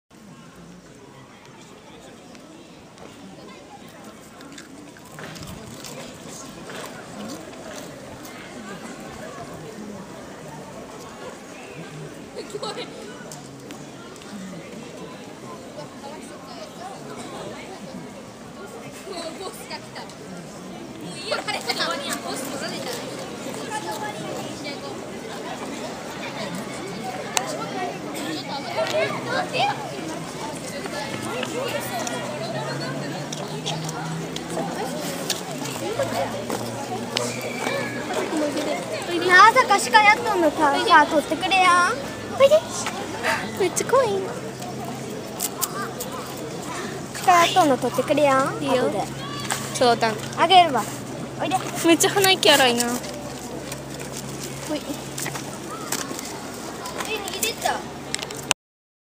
子鹿に鹿せんべいをあげてみる！＠奈良公園 その284
奈良公園の子鹿に、鹿せんべいをあげてみました。